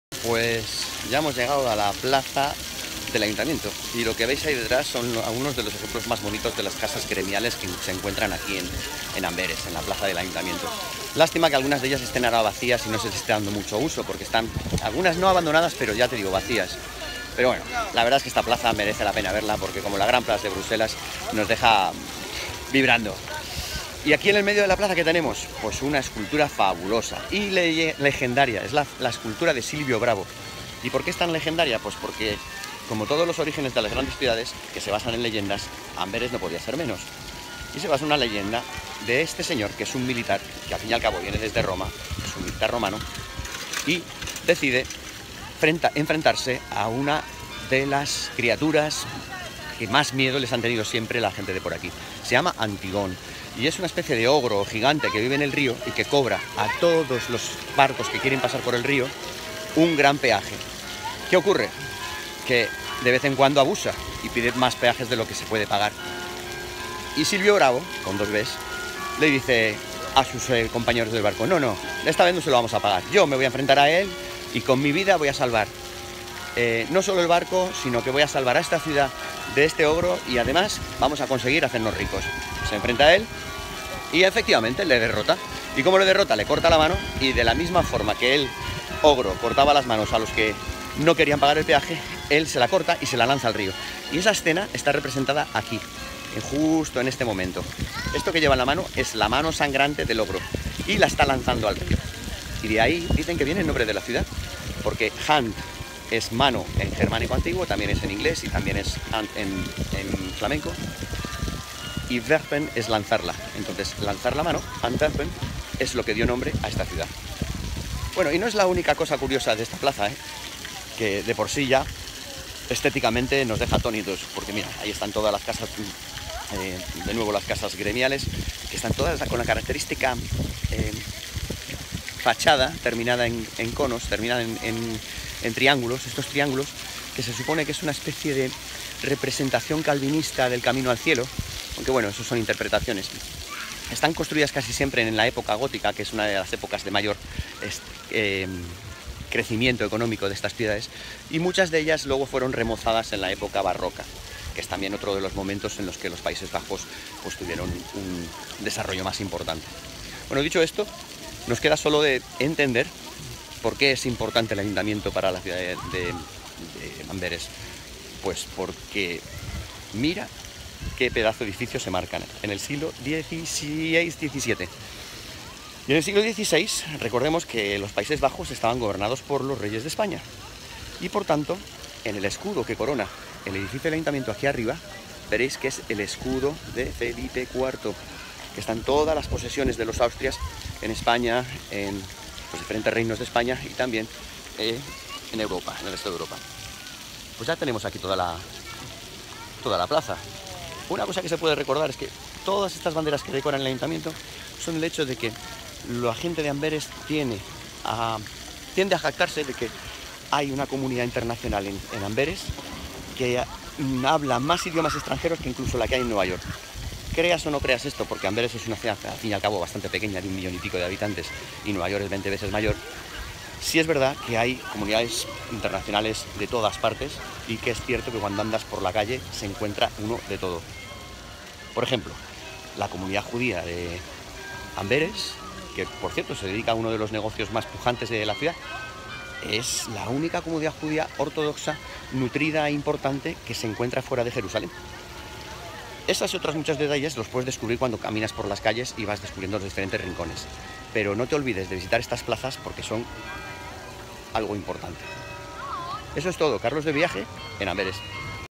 AMBERES - Grote Markt
La Grote Markt, el Ayuntamiento, la leyenda y fuente de Sylvio Brabo y las Casas Gremiales de Amberes / Antwerpen